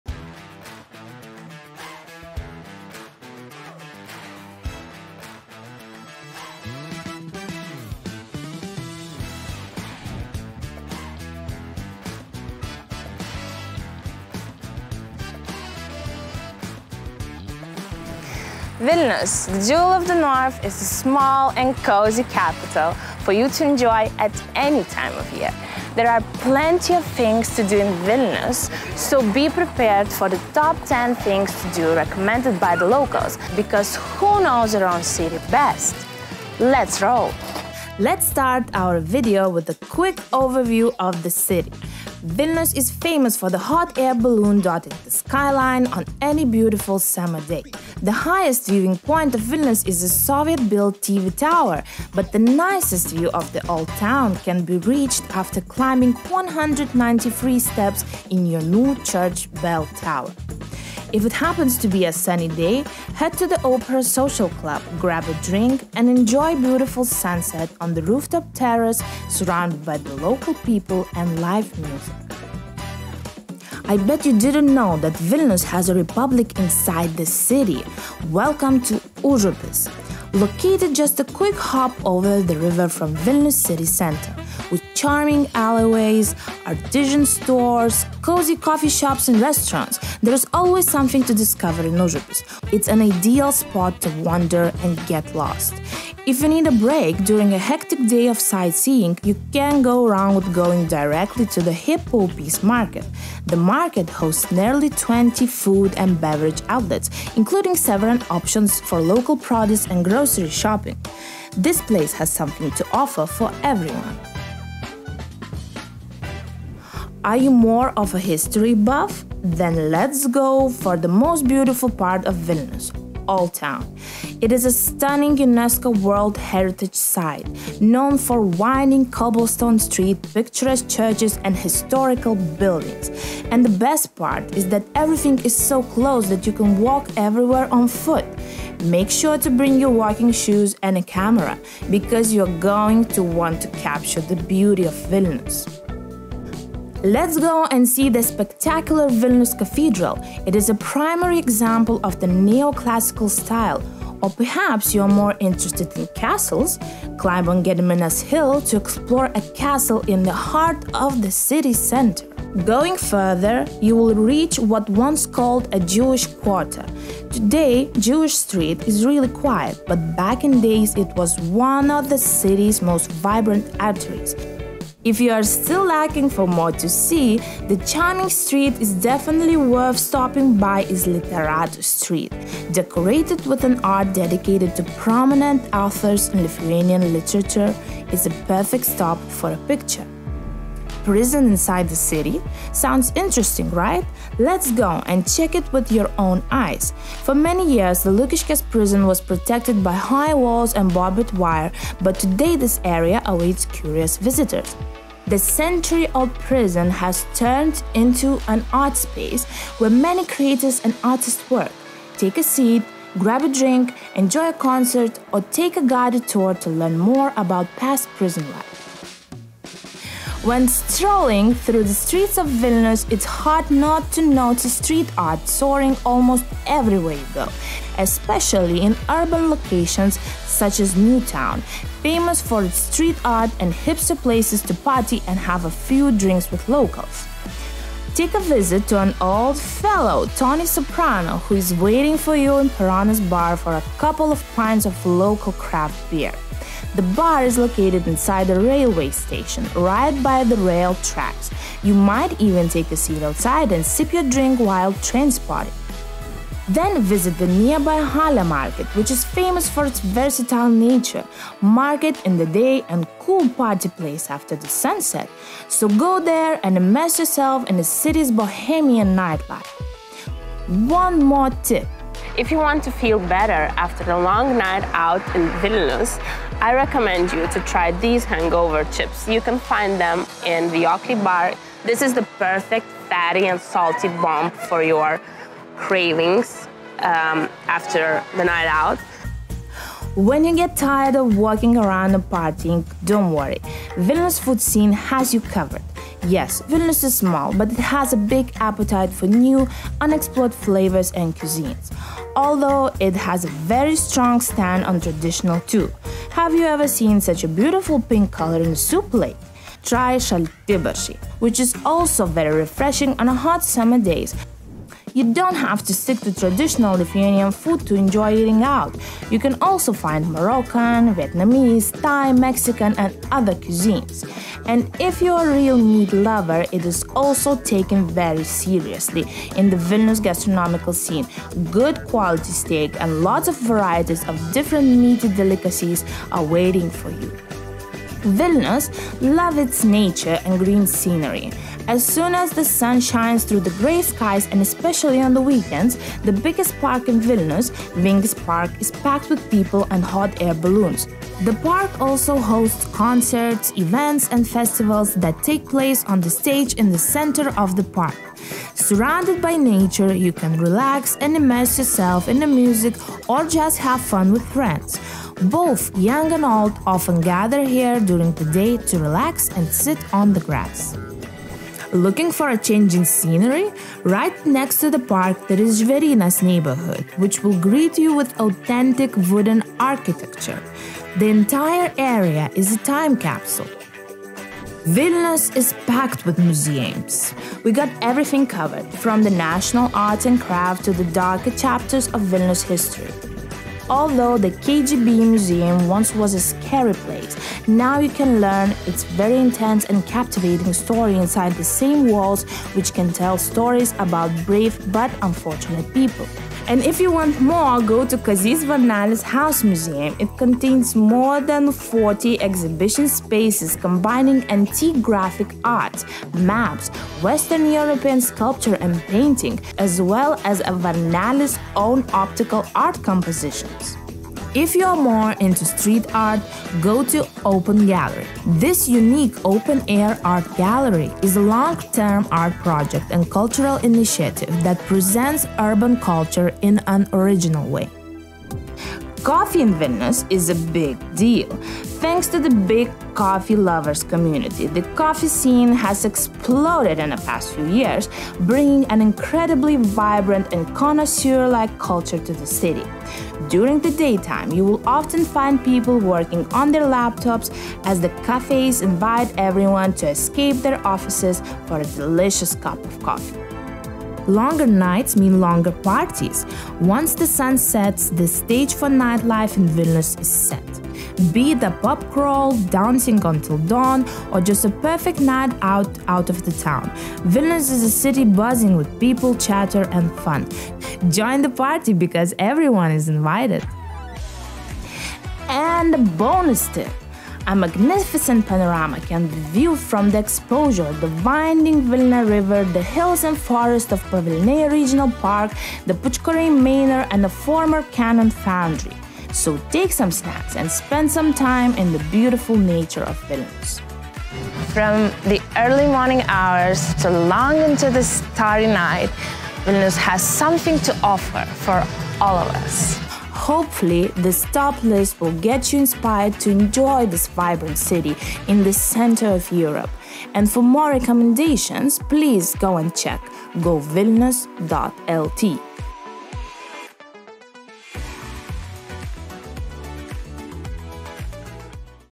Top 10 things to do in Vilnius
Whether you’re an avid traveler ready to embark on your next big adventure or a talented professional looking to take your career to the next level, we’re here to help you make the most of your time in Vilnius.

0:00 - Intro
0:39 - Vilnius from above
1:15 - Užupis district
1:58 - Explore the old town and find a magic of it
3:11 - Lukiškės prison
3:41 - New town
4:52 - Get your plate ready for unique foods
5:39 - Vingis park and Žvėrynas neighbourhood
6:26 - Museums
7:20 - Artisan coffee and coffee culture
7:45 - Nightlife